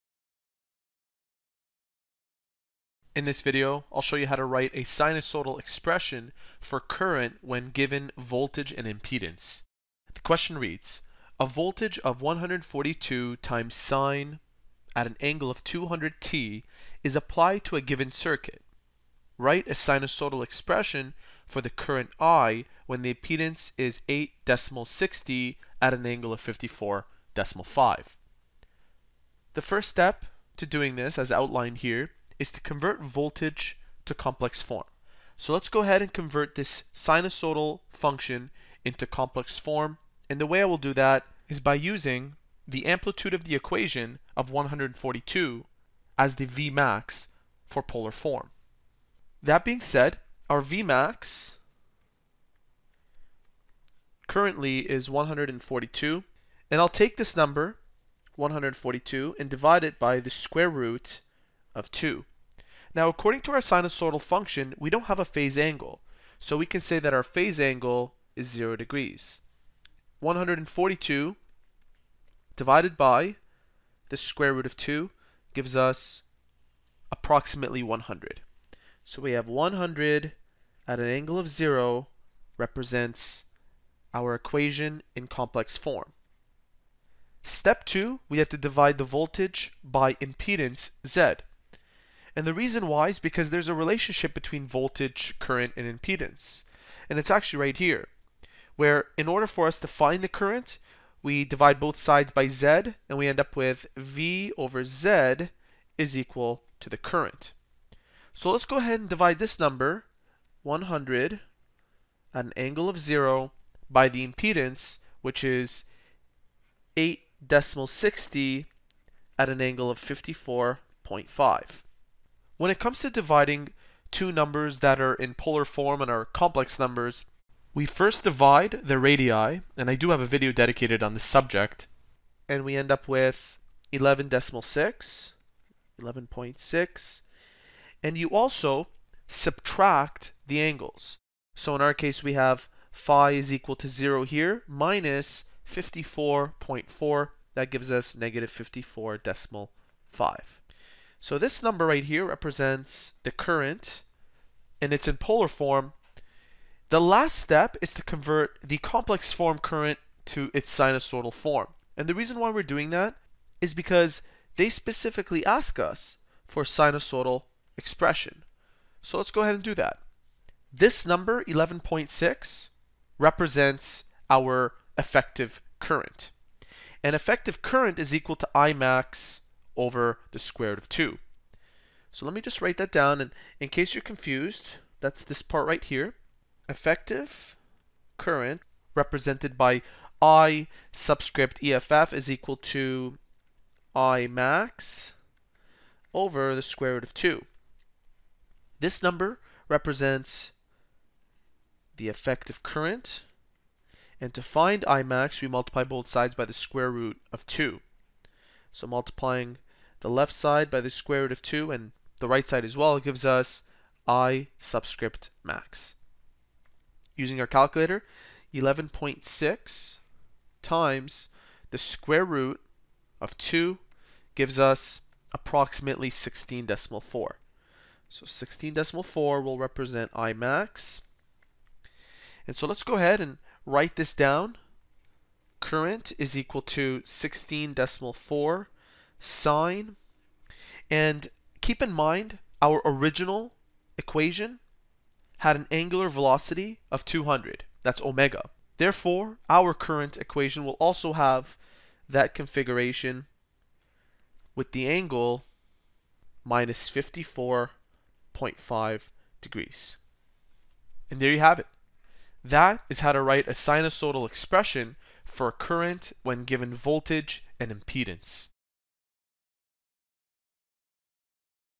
📚 How to write a sinusoidal expression for current when given voltage and impedance
Q.   A voltage of 142 sin⁡(200t)  is applied to a given circuit. Write a sinusoidal expression for the current i when the impedance is 8.60∠54.5°.

Step 1: Convert voltage to complex form (V).
Step 2: Divide voltage (V) by impedance (Z).
Step 3: Convert complex form current (I) to sinusoidal form i.

What you'll need:

Current is represented by:   (i=I_max  sin⁡〖(ωt+ϕ_2 )   ⇒  I=I_eff∠ϕ_2=I_max/√2∠ϕ_2 〗 )

Voltage is represented by:   (v=V_max  sin⁡〖(ωt+ϕ_1 )   ⇒   V=V_eff∠ϕ_1 〗=V_max/(√2)∠ϕ_1 )

Ohm's Law for AC:   ▭(V=IZ)